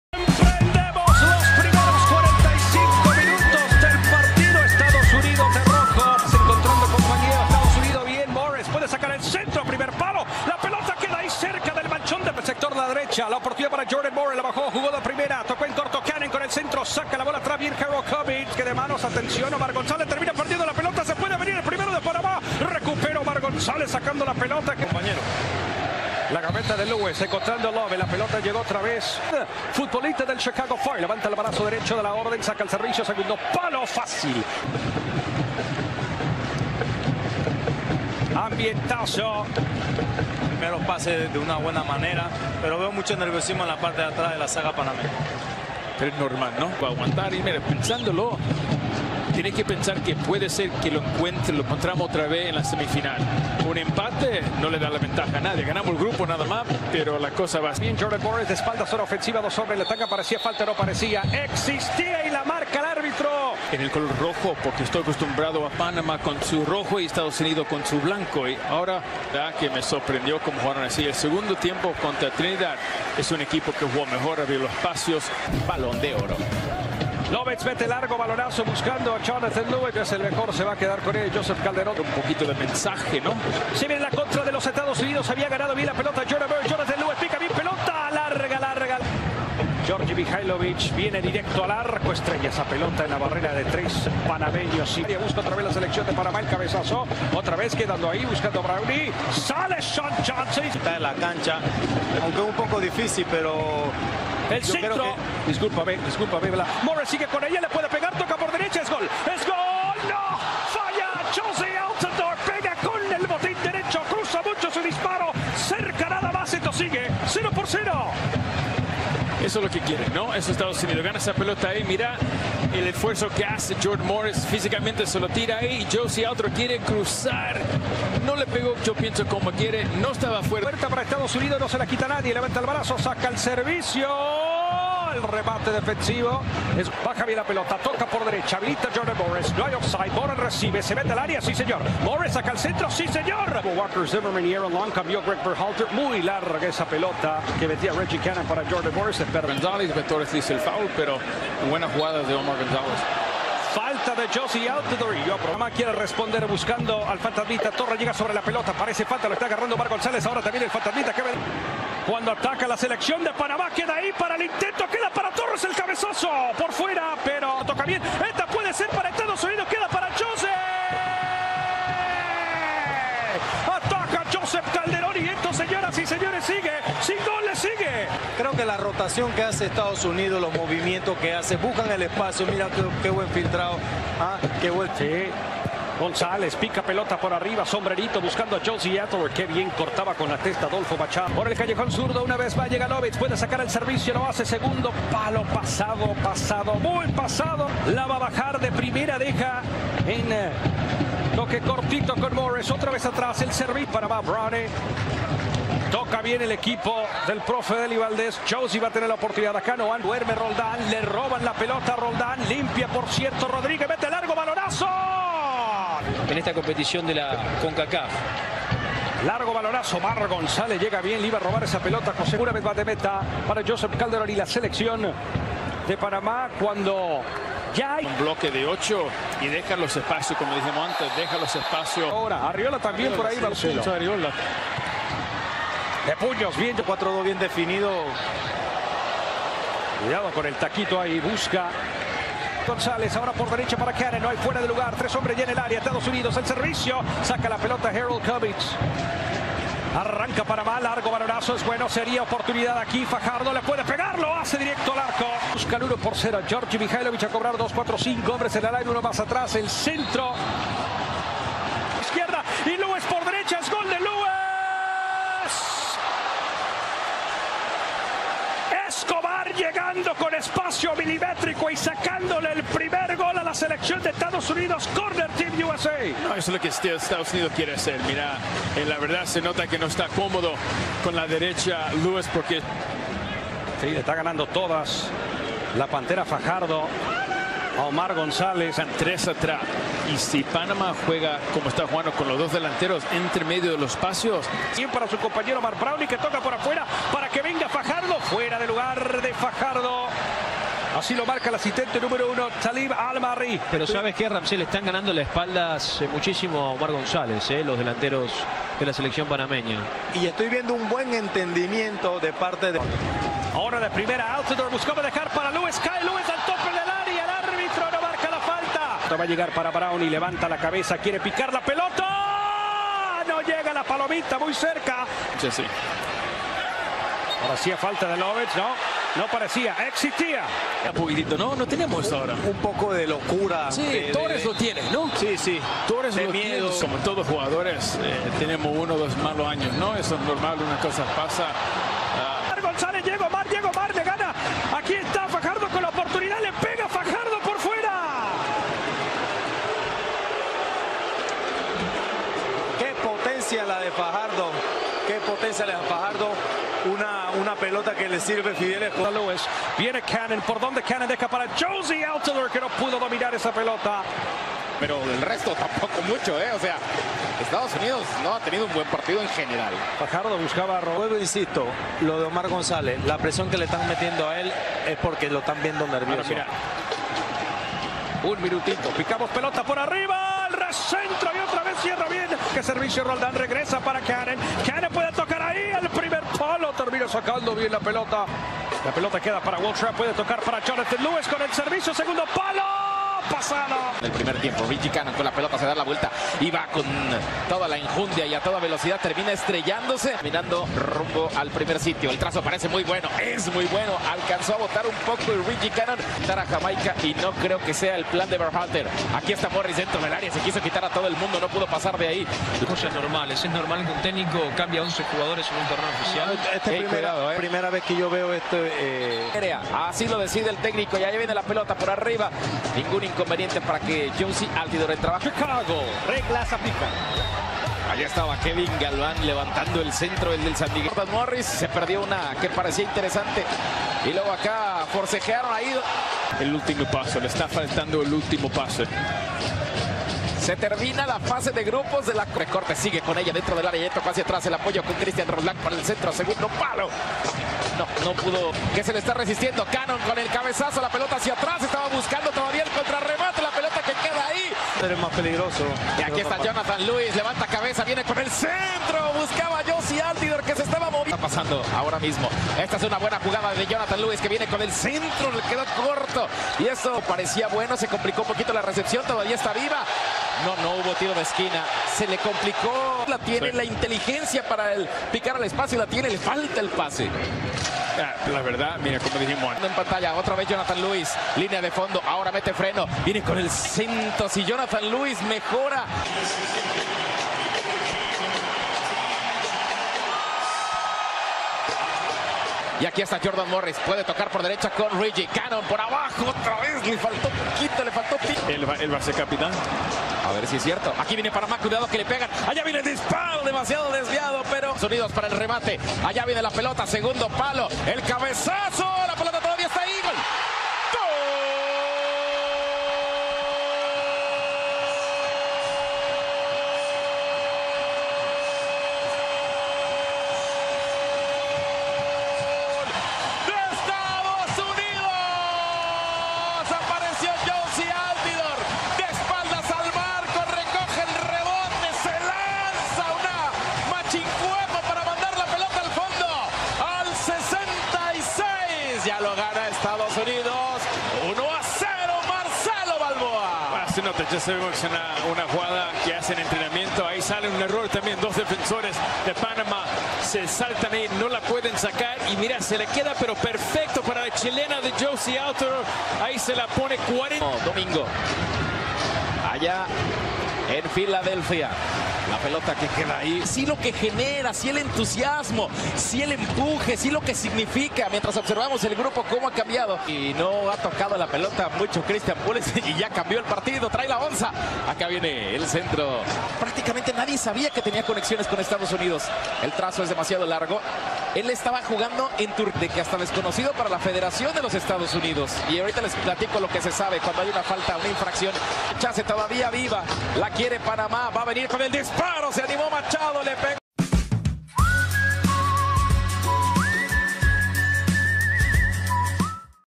USA X Panama 1-0 Highlights | Concacaf Gold Cup Group D | 27-06-2019
We Love Sports
Concacaf Gold Cup Group D